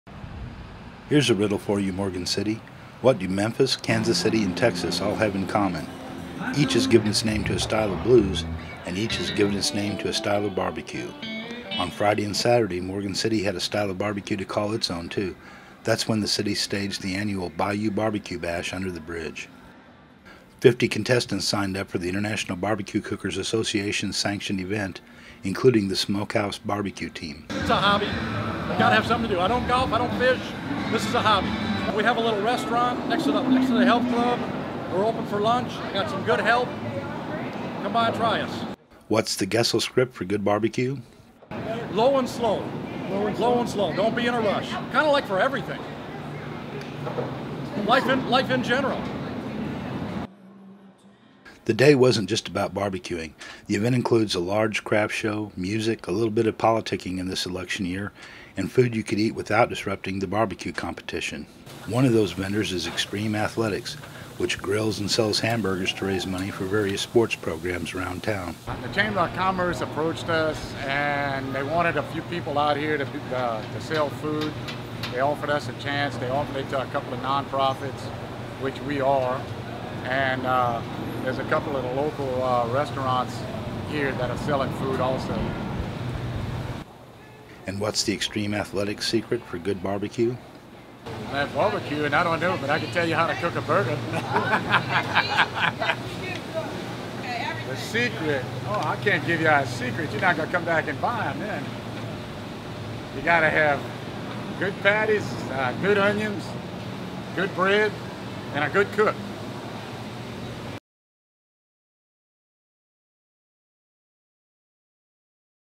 2015 Bayou BBQ Bash
Morgan City hosts an International Barbecue Cookers Association competition, plus a craft show, with food, drink and music.